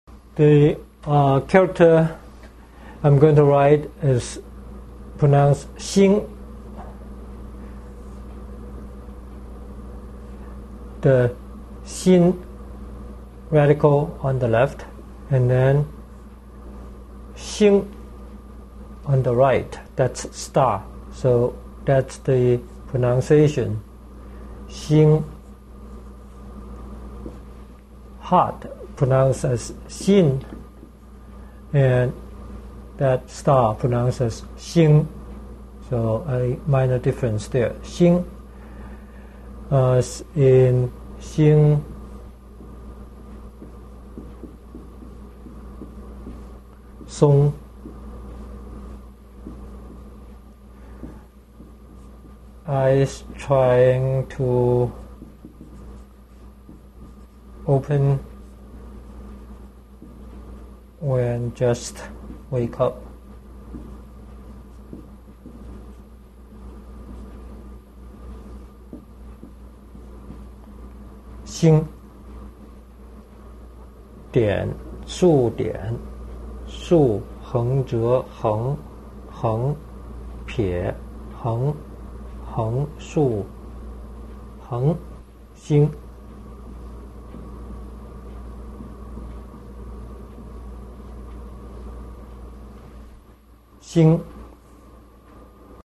Chinese Symbols for Waking Up
Chinese Symbols for Waking Up. Part of the series: Chinese Radicals Xin 1: Part 5. Learn Chinese symbols for "waking up" with tips on writing and pronouncing Chinese characters from a Chinese language teacher in this free video on Chinese words and phrases.